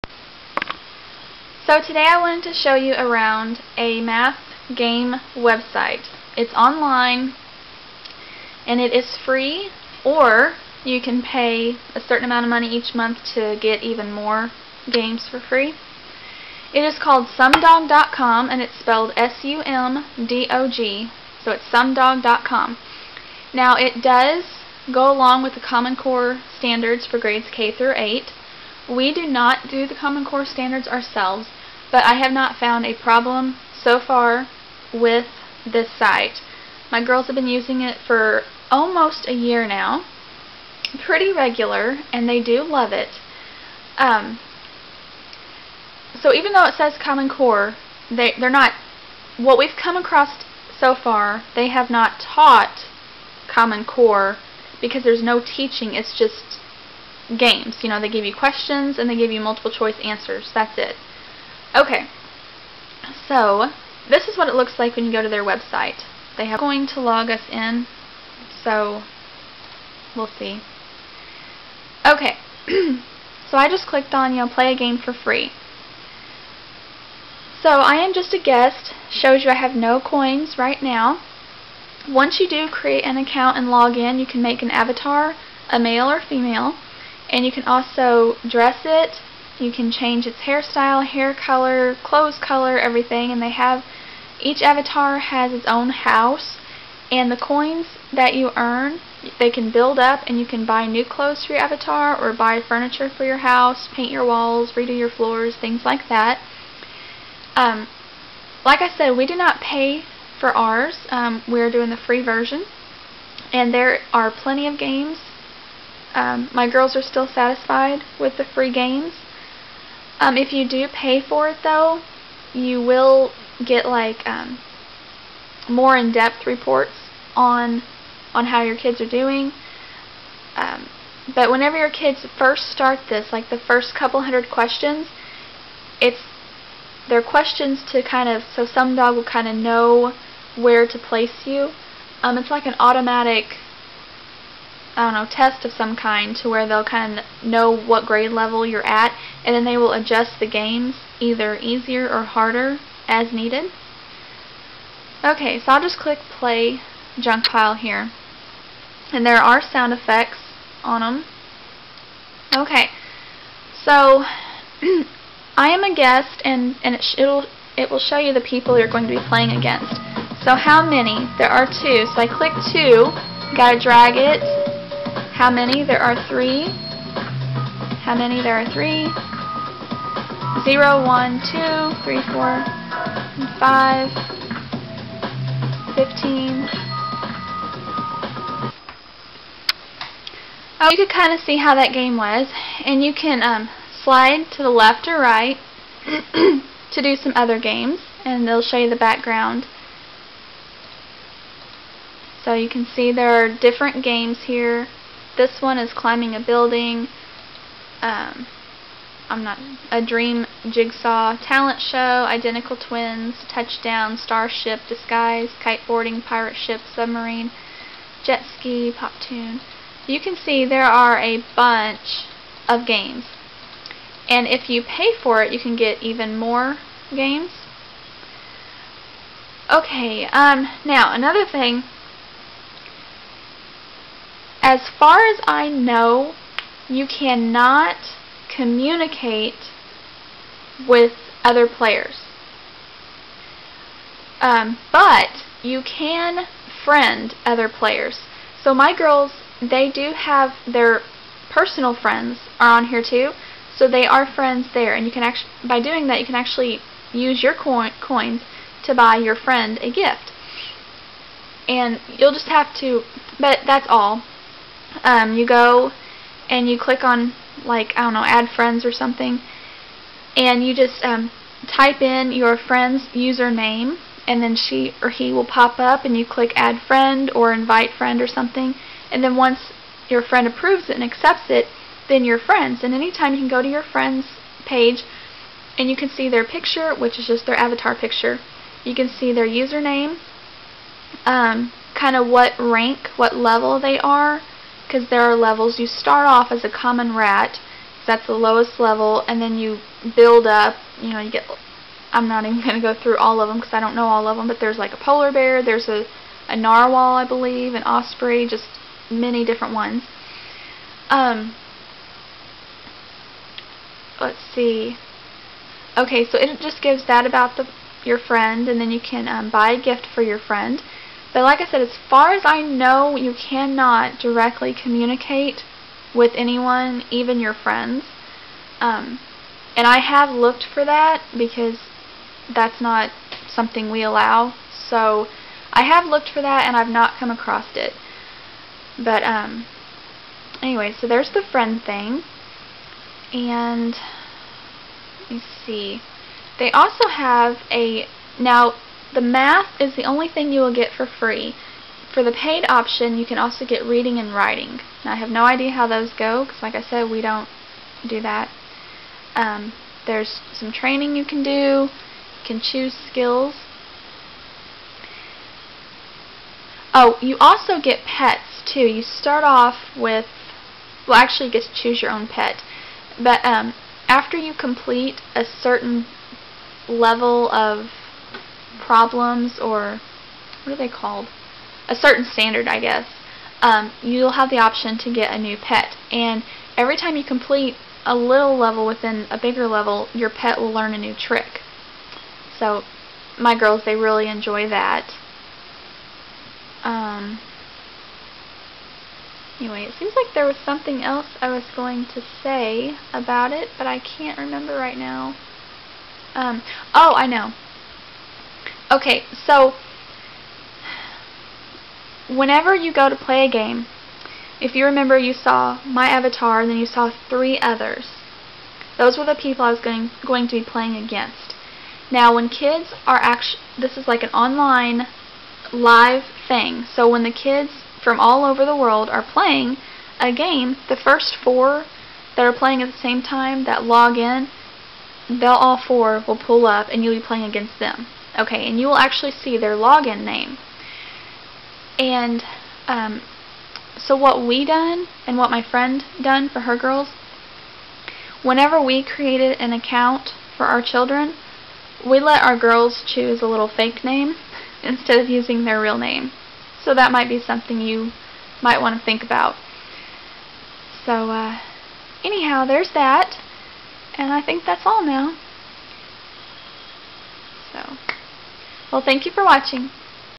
Website Review: Math: Sumdog-Online Math Game
This is a review over Sumdog.com. Sumdog is composed of online math games to help children master certain math skills. My kids absolutely love it. Since I made this video Sumdog has taken away MANY, if not MOST, of the FREE games, as of now they only offer 10 free games. So if you pay the price of $6/student/month you can get much more.